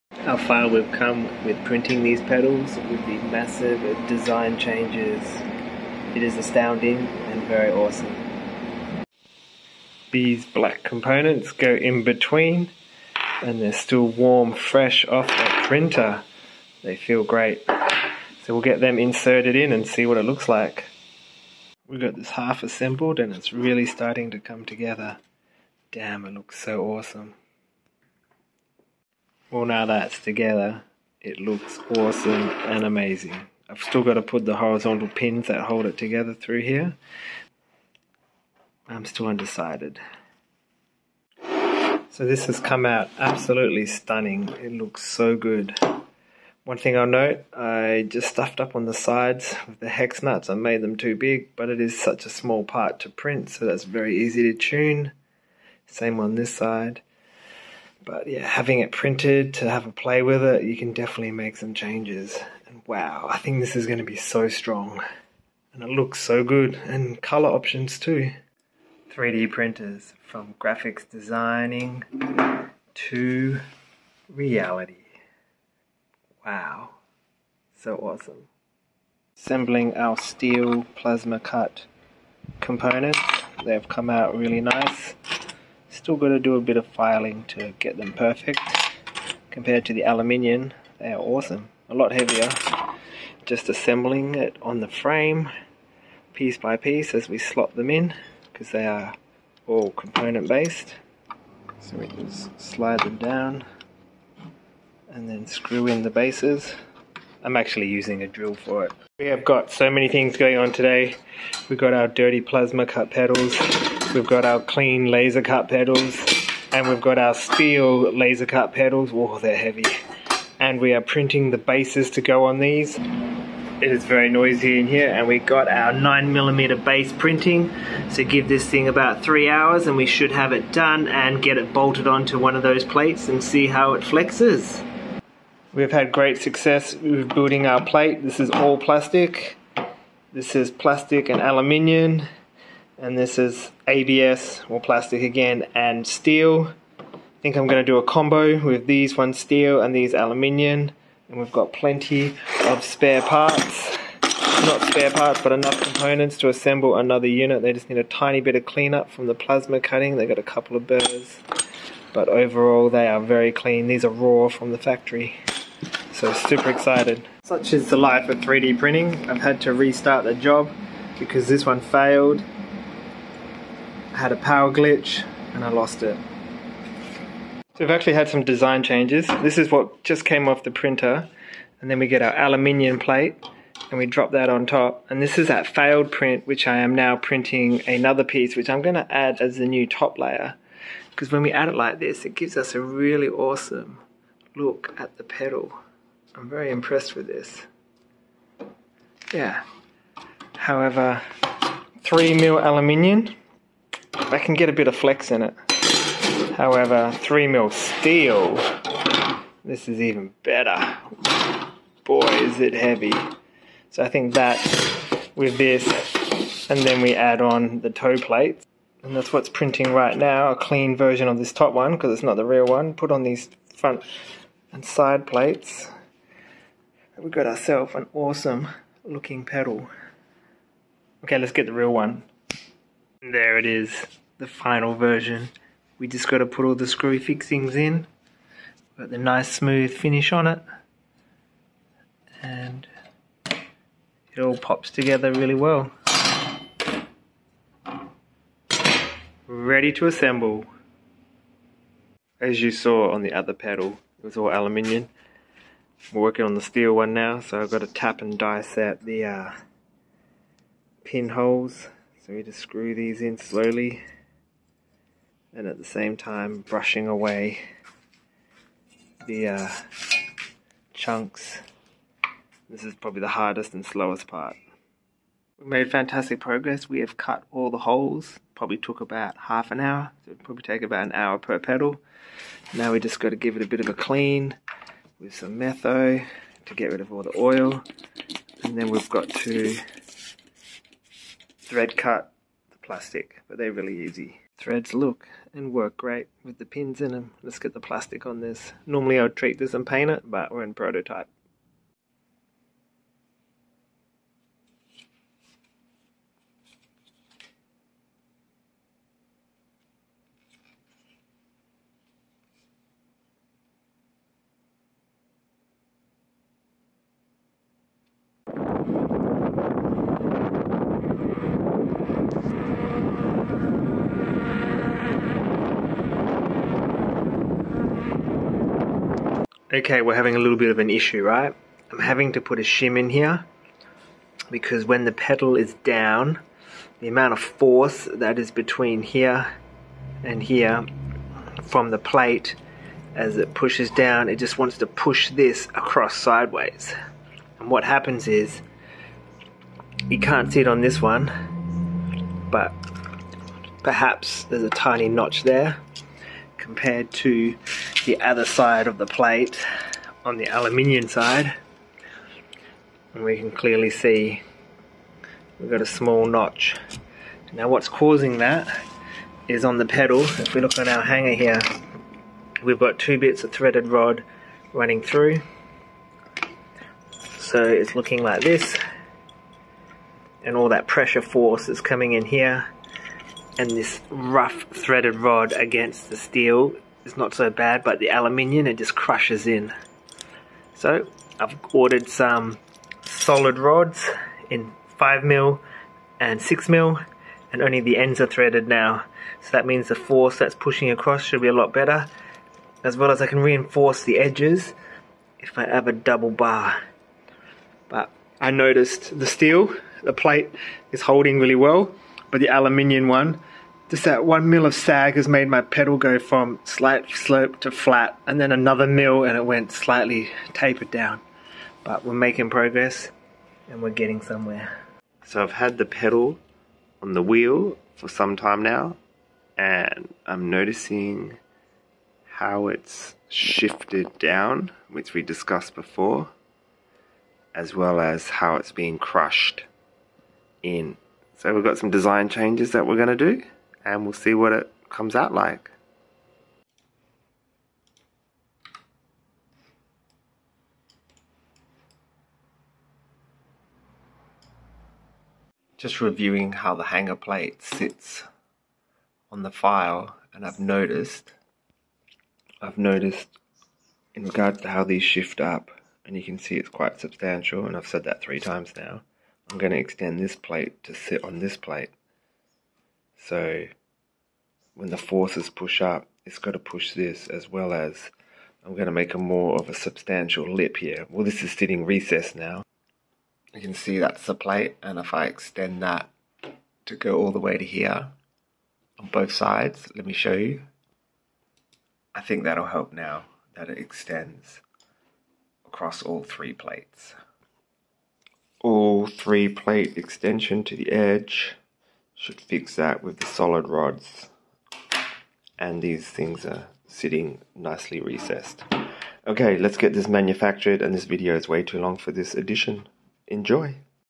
EUC 3D Printed Pedals Part 3 Laser cut mixed materials
And the journey continues on my 3D printed pedals I have started incorporating mixed materials of aluminum and steel
it's not about the cost it's about the journey